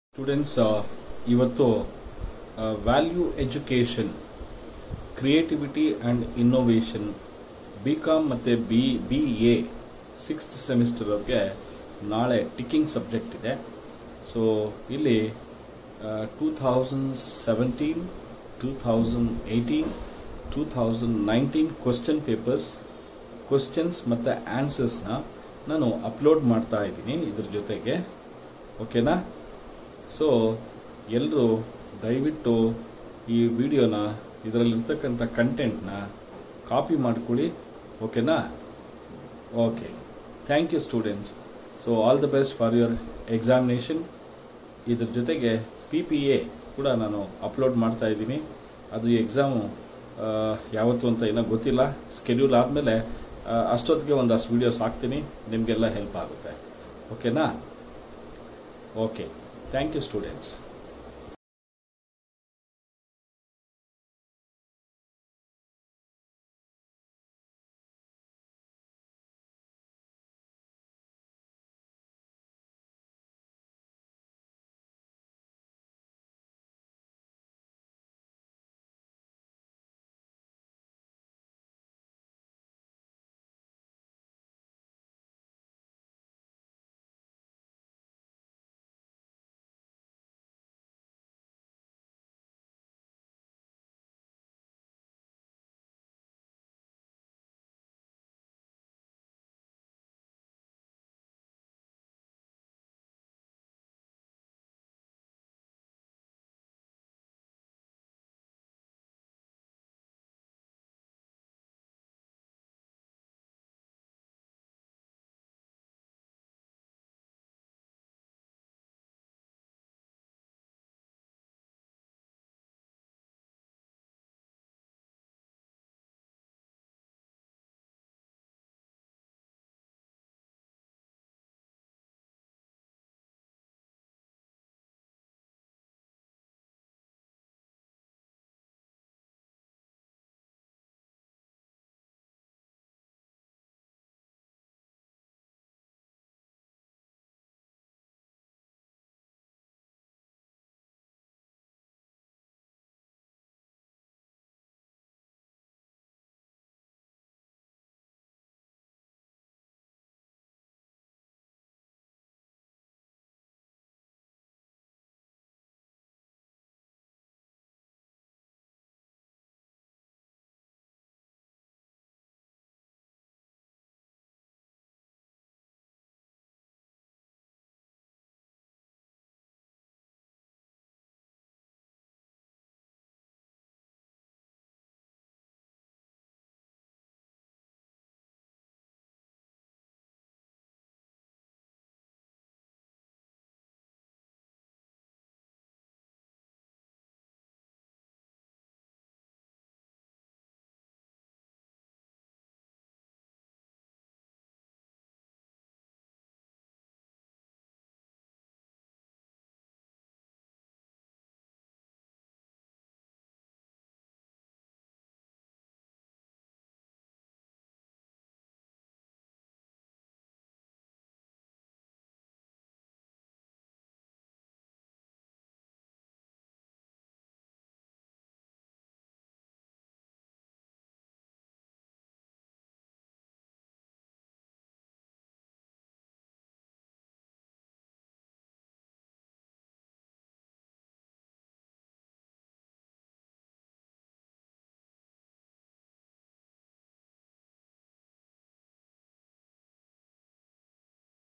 Value Education - Creativity and Innovation for B.Com and B.B.A 6th Semester - 2017 to 2019 QP
Value Education - Creativity and Innovation for B.Com and B.B.A 6th Semester - 2017 to 2019 QP- SRINATH SIR